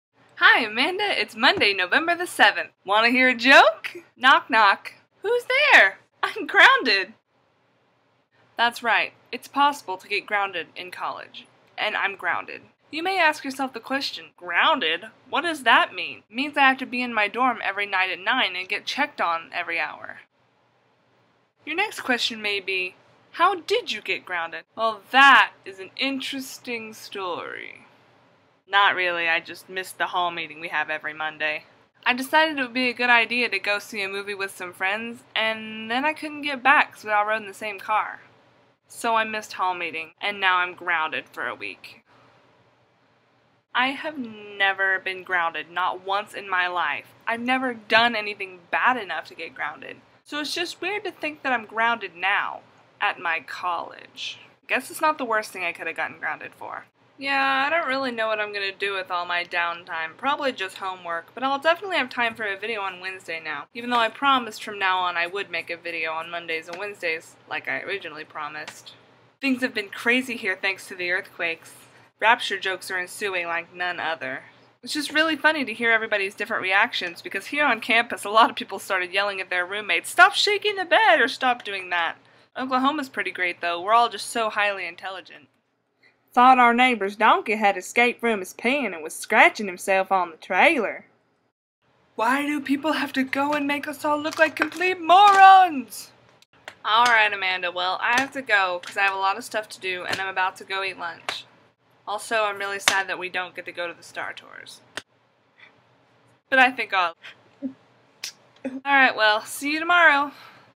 Earthquakes and Groundings
Where I elaborate on the potential of getting grounded on campus, and the fact that we just had several earthquakes in the span of a few days!